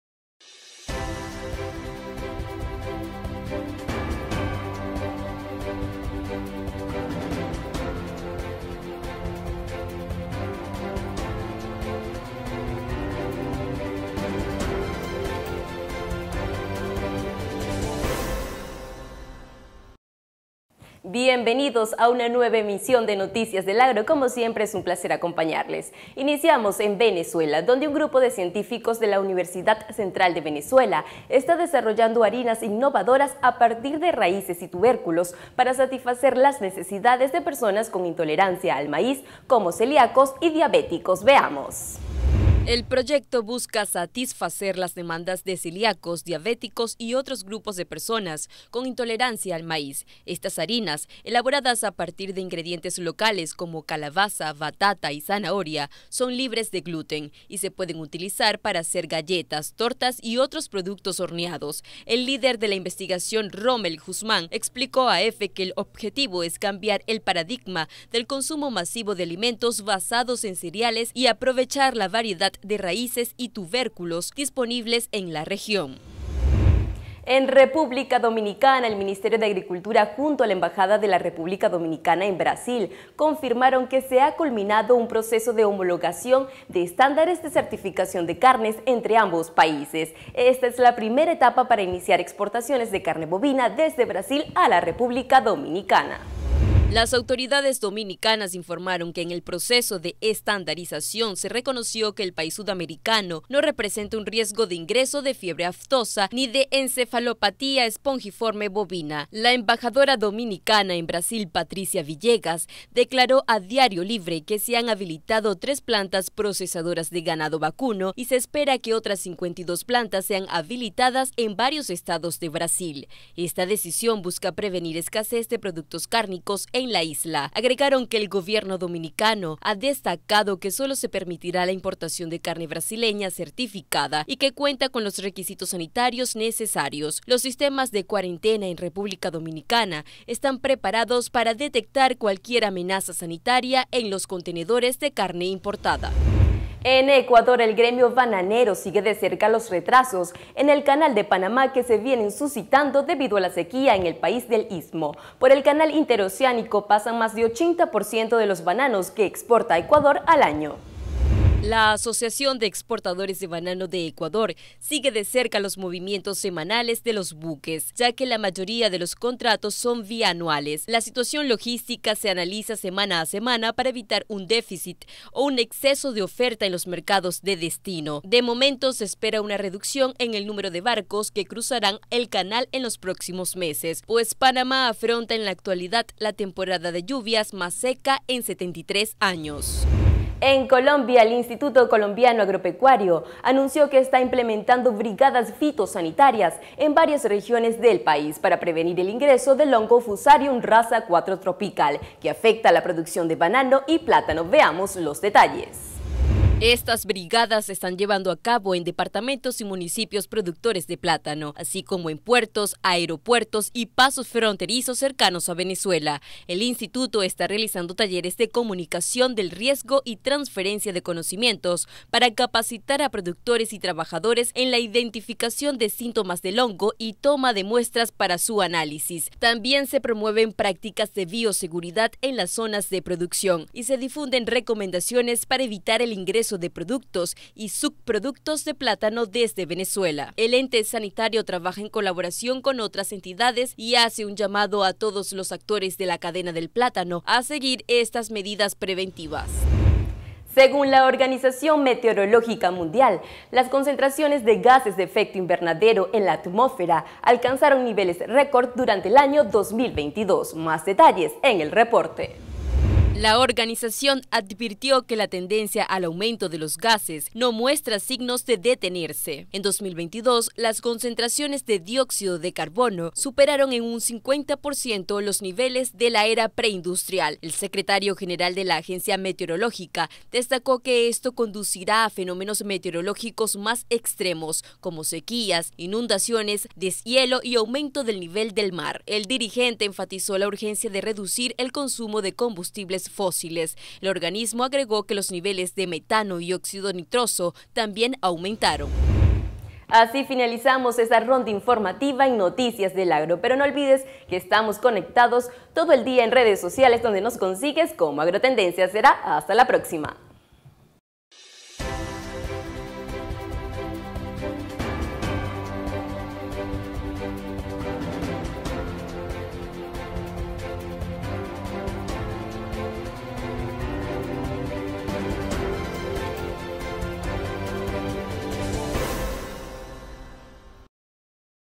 🔴 Desarrollan harina a base de tubérculos en Venezuela + NDA
-Desarrollan harina a base de tubérculos en Venezuela
-República Dominicana inició gestiones para importar carne desde Brasil
Gremio bananero de Ecuador monitorea retrasos en el canal de Panamá
-Colombia activó dispositivo de prevención contra el Foc R4T en fronteras
-La ONU alertó sobre aumento de gases de efecto invernadero

Encuentra lo que necesitas para tu unidad de producción en: Agroshow, la expo del agro